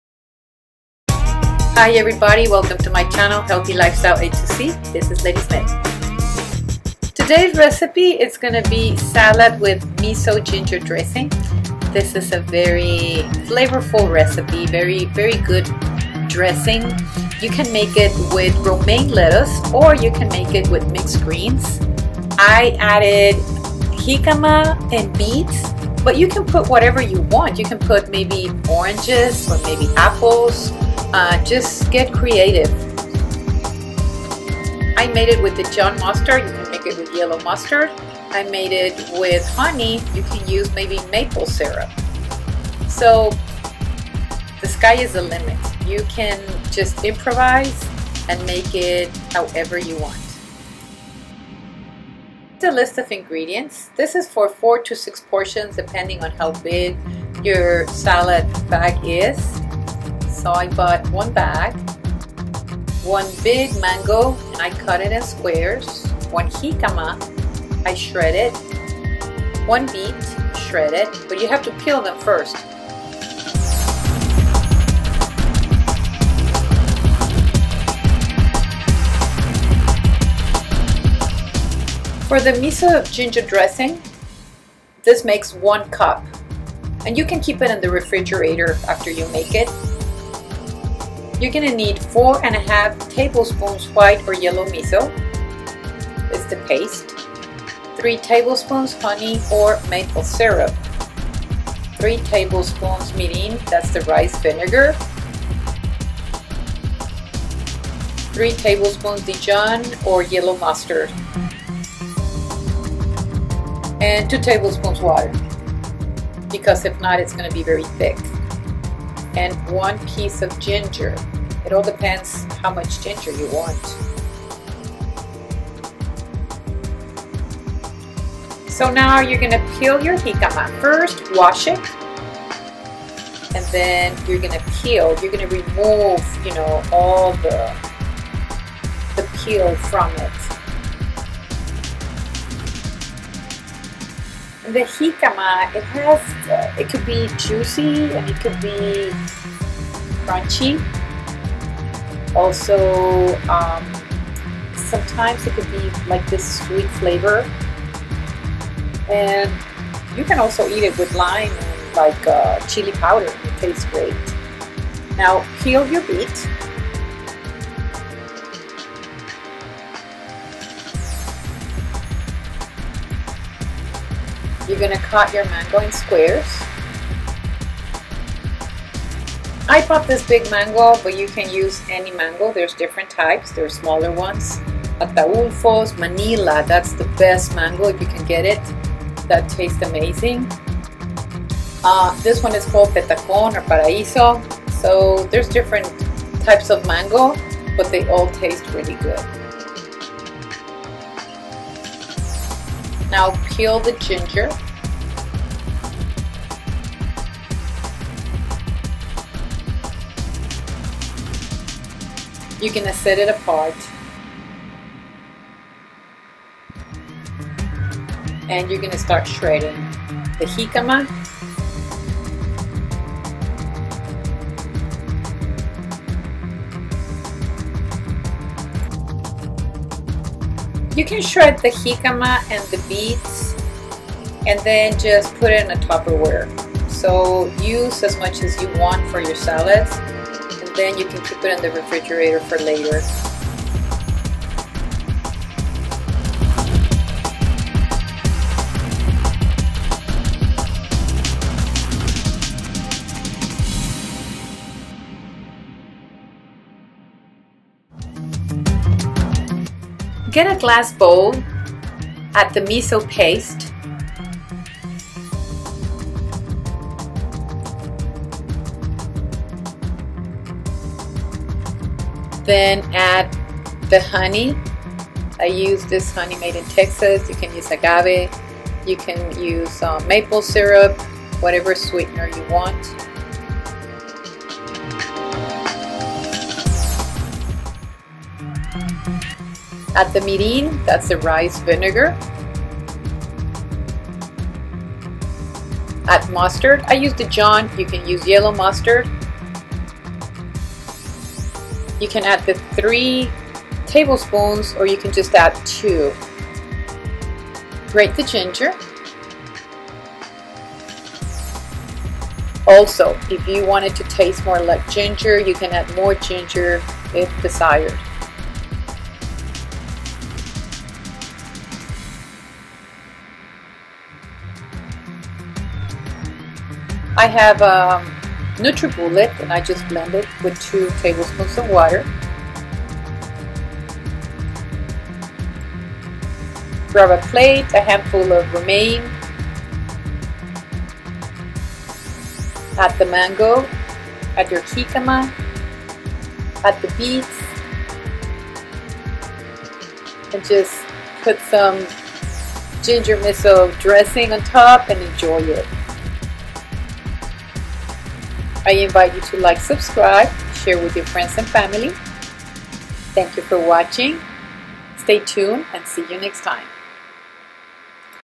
SALAD WITH MISO GINGER DRESSING/ENSALADA CON ADEREZO DE MISO Y JENGIBRE
FLAVORFUL SALAD WITH MANGO, JICAMA AND BEET WITH A DELICIOUS MISO GINGER DRESSING.
 SABROSA ENSALADA CON CON MANGO, JICAMA Y BETABEL CON UN DELICIOSO ADEREZO DE MISO Y JENGIBRE.
RECIPE
SALAD WITH MISO GINGER DRESSING
4 – 6 PORTIONS

INGREDIENTS
SALAD
1 BAG ROMAINE LETTUCE OR MIXED GREENS
1 JICAMA PEELED AND SHREDDED
1 BEET PEELED AND SHREDDED
1 BIG MANGO OR ANY MANGO.

DRESSING
4 ½ TBSP MISO WHITE OR YELLOW
3     TBSP MIRIN RICE VINEGAR
4 ½ TBSP HONEY, AGAVE OR MAPLE SYRUP
2 OR 3 TBSP DIJON OR YELLOW MUSTARD
1 PIECE OF GINGER
2 TBSP WATER

INSTRUCTIONS

MAKE THE DRESSING, MIXING ALL THE INGREDIENTS IN A BLENDER.
CUT MANGO IN SQUARES, PEEL AND SHRED JICAMA AND BEET.
PLACE A HANDFULL OF SALAD ON A PLATE, ADD THE MANGO, JICAMA AND BEET. POUR SOME DRESSING ON TOP.

ENSALADA CON ADEREZO DE MISO Y JENGIBRE
4 - 6 PORCIONES

INGREDIENTES
ENSALADA
1 BOLSA DE LECHUGA ROMANA O LECHUGA MIXTA
1 JÍCAMA PELADA Y RALLADA
1 BETABEL PELADO Y RALLADO
1 MANGO GRANDE O CUALQUIER MANGO EN CUADRADOS

ADEREZO
4 ½ CUCHARADAS DE MISO BLANCO O AMARILLO
3 CUCHARADAS DE VINAGRE DE ARROZ MIRIN
4 ½ CUCHARADAS DE MIEL, AGAVE O JARABE DE MAPLE
2 o 3 CUCHARADAS DE DIJON O MOSTAZA AMARILLA
UN TROZO DE JENGIBRE RALLADO
2 CUCHARADAS DE AGUA

INSTRUCCIONES
HACER EL ADEREZO, MEZCLANDO LOS INGREDIENTES EN UNA LICUADORA.
CORTAR EL MANGO EN CUADRADOS, PELAR Y RALLAR LA JÍCAMA Y EL BETABEL.
PONER UN PUÑADO DE ENSALADA EN UN PLATO, AÑADIR EL MANGO, LA JÍCAMA Y EL BETABEL. VIERTE UN POCO DE ADEREZO ENCIMA.
¡A DISFRUTAR!